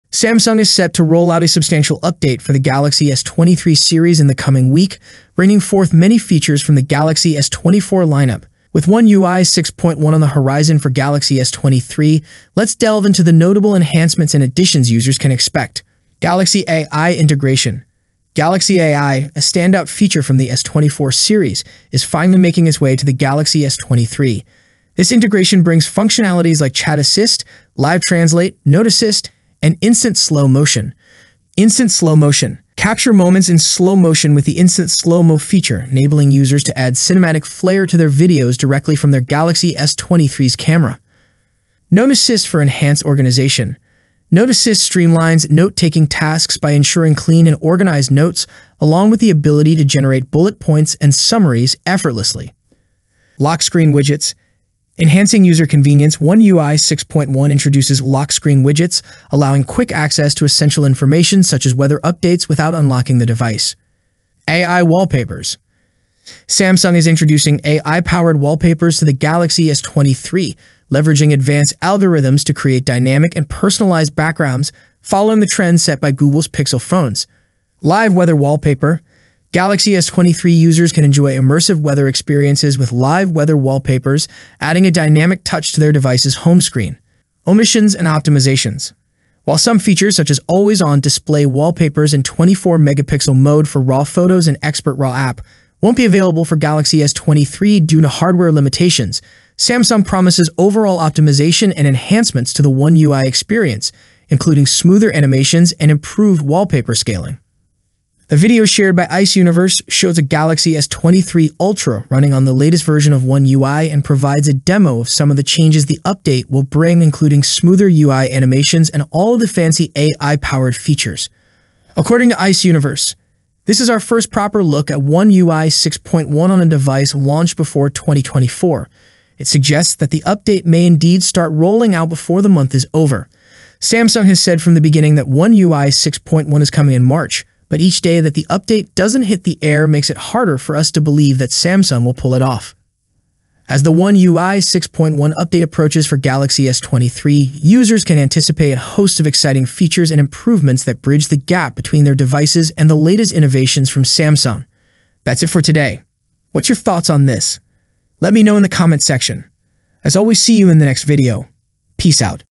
MAJOR ONE UI 6.1 For Samsung Galaxy S23 Ultra - New AI Features!
Finally, ONE UI 6.1 For Samsung Galaxy S23 Ultra is Here. You can also check out One UI 6.1 in action on the Galaxy S23 Ultra with the courtesy of Ice Universe.

PEACE!!